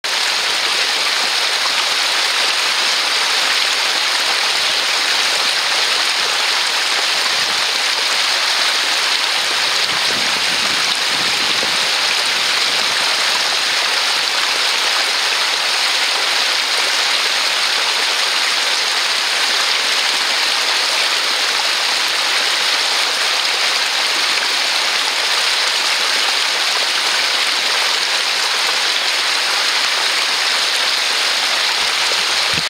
Sonido gotas agua
Sonido bajada de agua hacia el arroyo de Oville, León